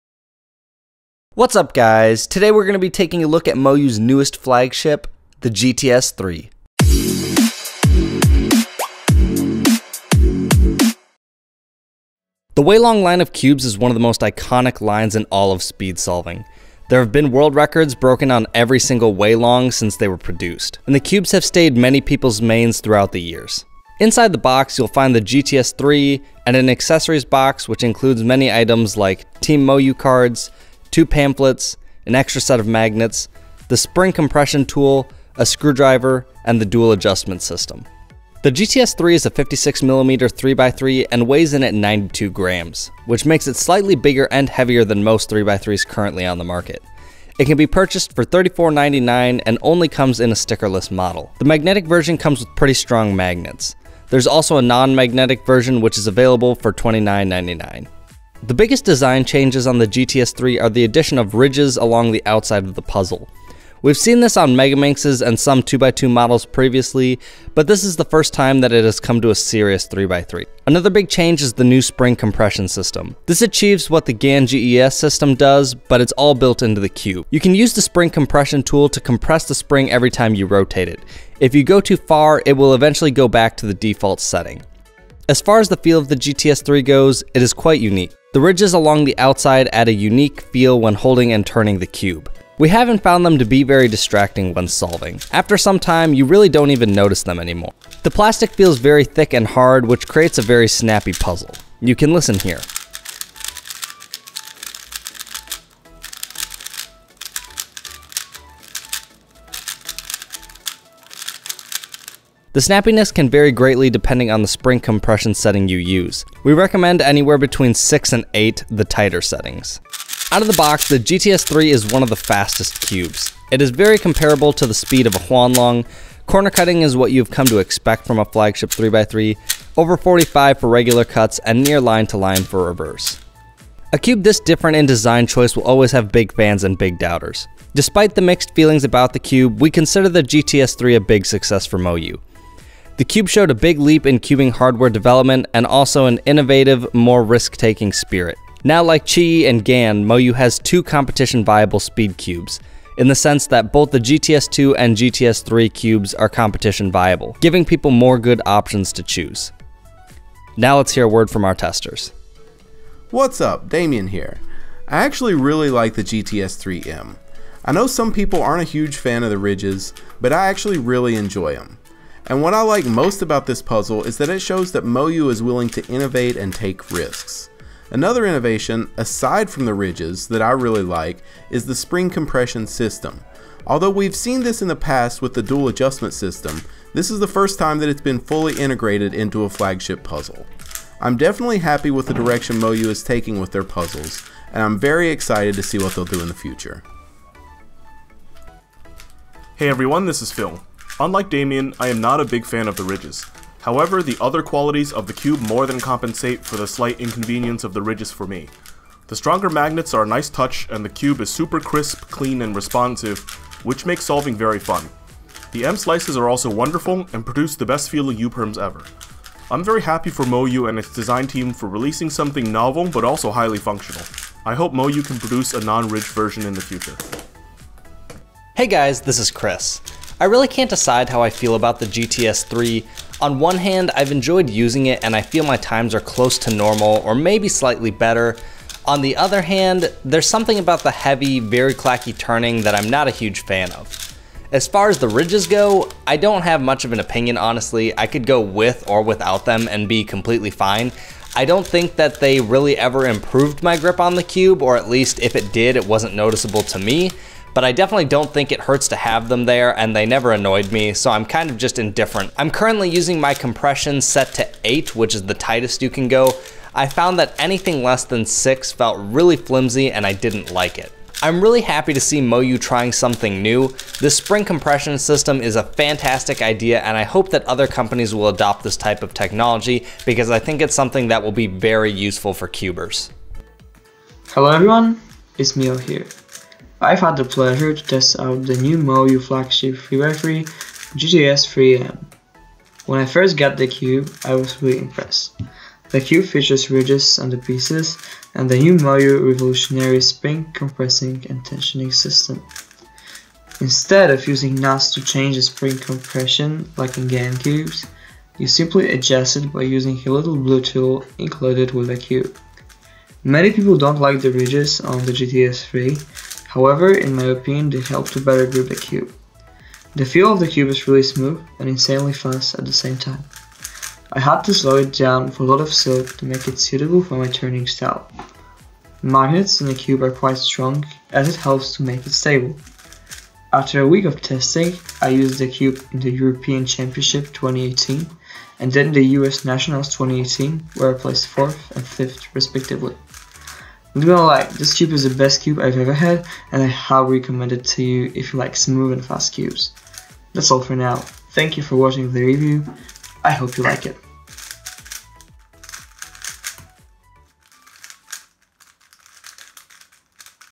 MoYu WeiLong GTS3 M In Depth Review (Featuring Michał Rzewuski)
This is our in depth review of the MoYu WeiLong GTS3 M, a new 3x3 with a wonderfully creative design. In addition to the outer ridges that aid with grip, the GTS3 comes with a spring compression gear system that allows for quick adjustments of spring elasticity on your cube. Definitely one of the most innovative and fun cubes recently released!

Many thanks to Michał Rzewuski for sharing his opinion on this cube! To see more content from him, go

Also follow him on Instagram at @michalrzew